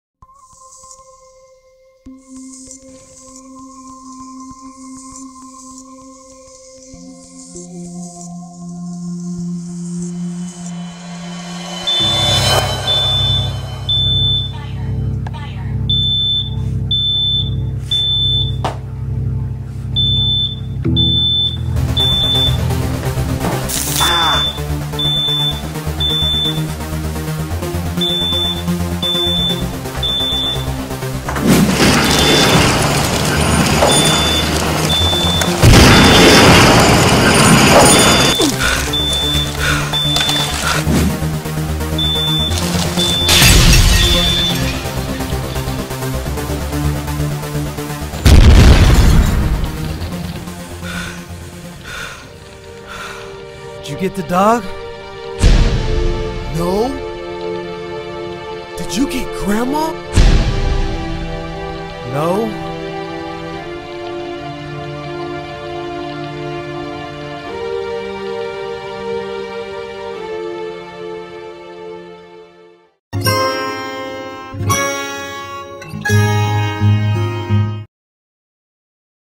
What to Grab When the House is on Fire
Mock Apple Commercial Produced and Edited by Craig Scuilla & Jason Torres.